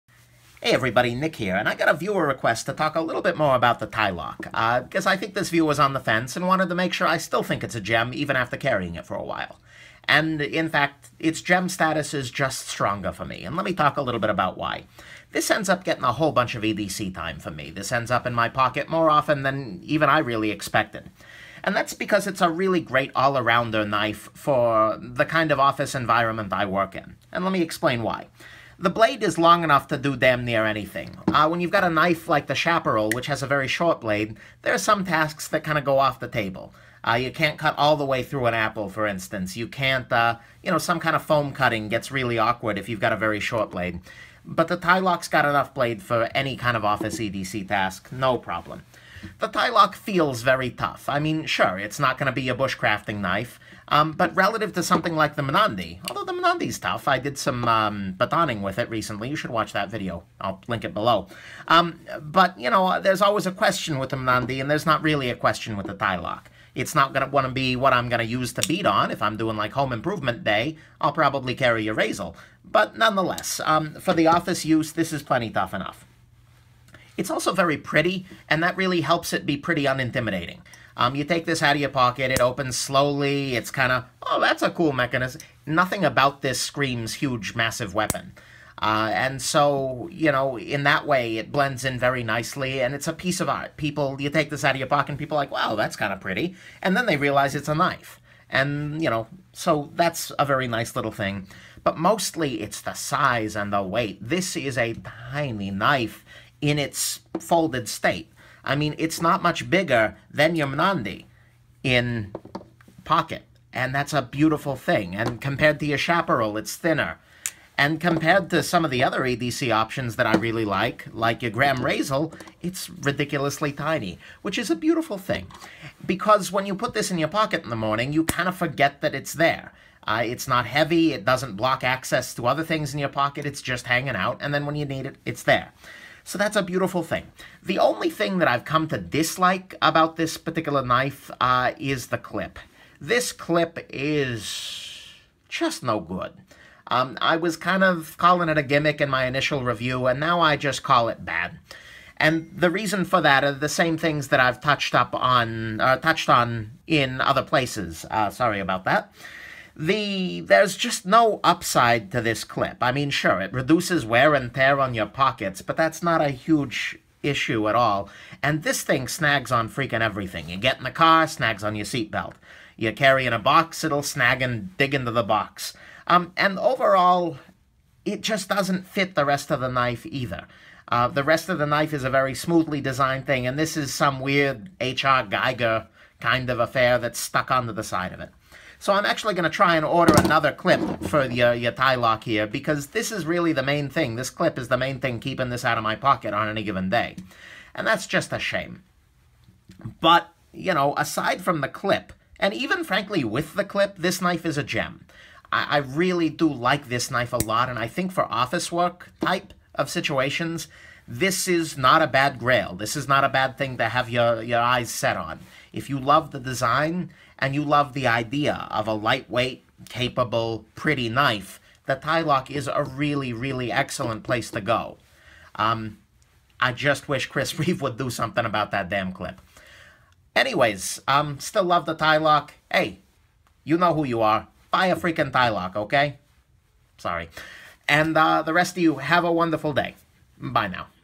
Chris Reeve TiLock: The Nick Shabazz Followup Review
A viewer asked me what I was thinking about my TiLock after giving it a bunch more EDC pocket time, so, here you go.

Quick summary?  Everything but the clip is an absolute gem